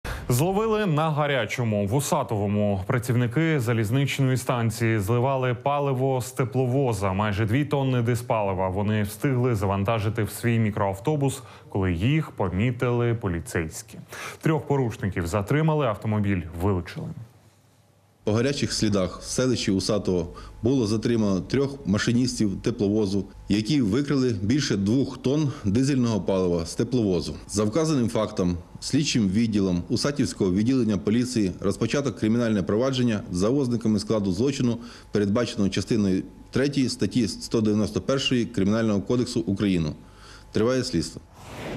Працівники залізничної станції в Усатово зливали паливо
Зловили на гарячому. В Усатовому працівники залізничної станції зливали пальне з тепловоза. Майже дві тонни дизпалива вони встигли завантажити у свій мікроавтобус, коли їх помітили поліцейські.

Трьох порушників затримали, автомобіль — вилучили.

«По гарячих слідах в селищі Усатове було затримано трьох машиністів тепловоза, які викрали більш ніж три тонни палива. За вказаним фактом слідчим відділом Усатівського відділу поліції розпочато кримінальне провадження, передбачене частиною третьою статті 191 Кримінального кодексу України. Триває слідство», — повідомив начальник Усатівського відділення поліції Ігор Панченко.